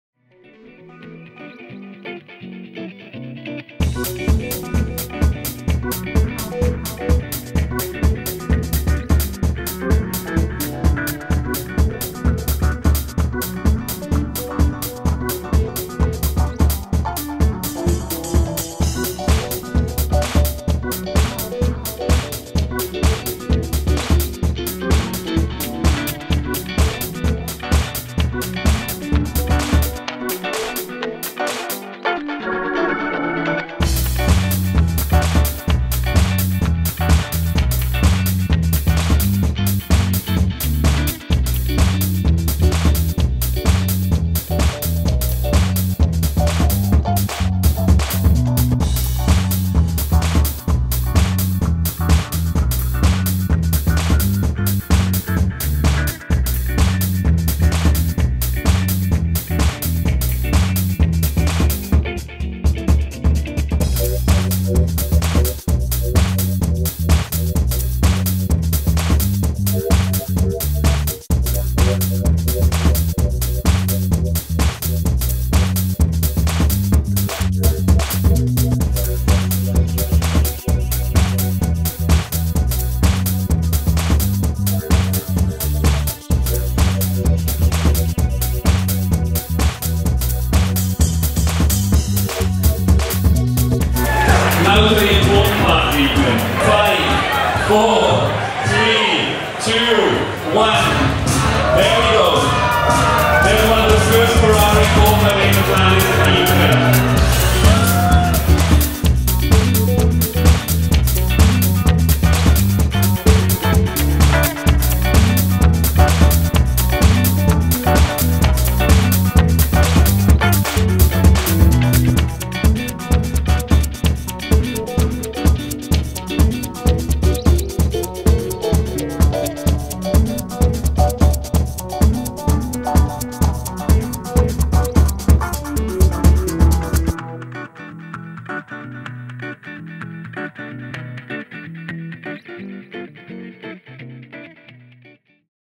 écurie25 Ferrari 458 Italia Launch Party
écurie25 celebrate the arrival of their two Ferrari 458 Italias in style. Over 500 guests joined in with the party in July of 2010. before even Ferrari had thrown their launch party!